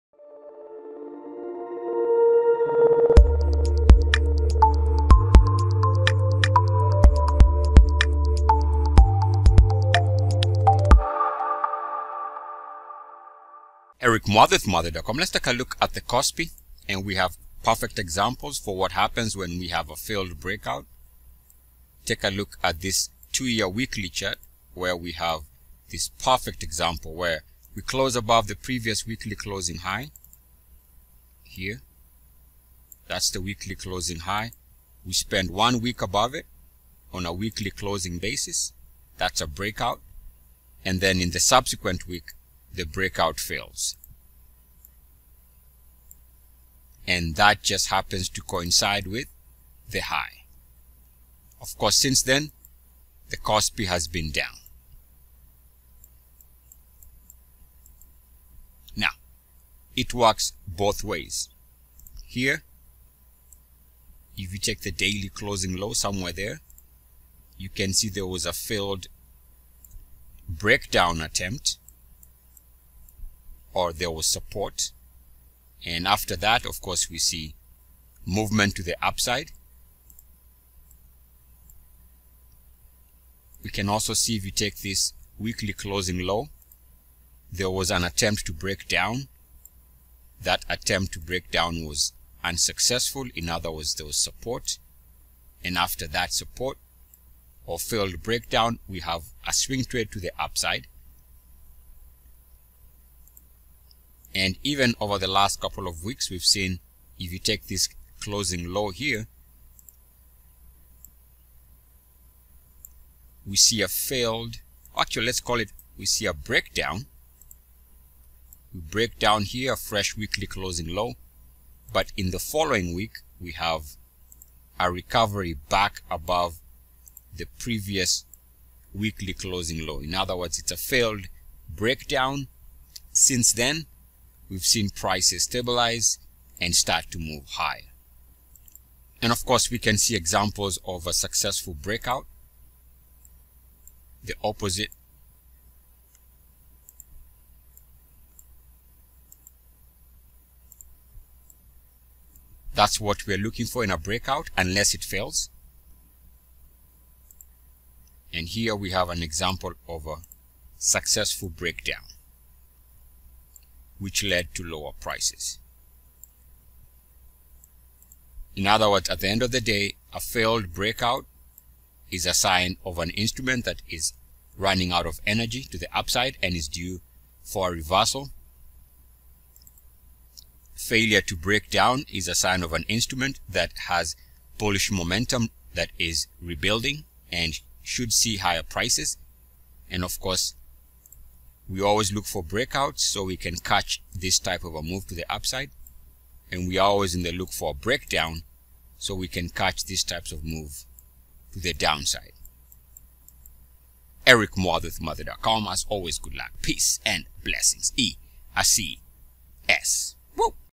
Price Breakout / Breakdown Success, or Failure - KOSPI - #813
We take a look at consequences of failed breakouts, and  negative divergence.

Bank of America Corp
NYSE: BAC $BAC